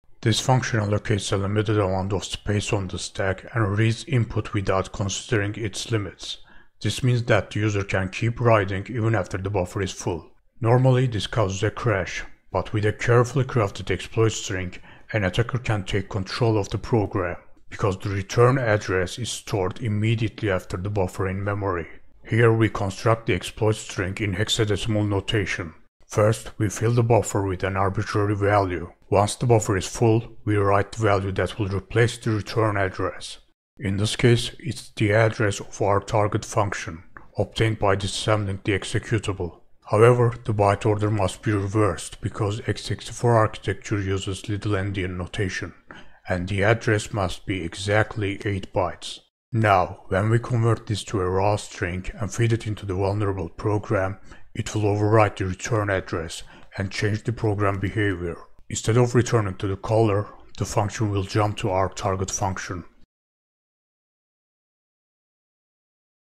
Exploiting Buffer Overflow: How Attackers Take Control
Focuses on the basics of buffer overflow and overwriting the return address.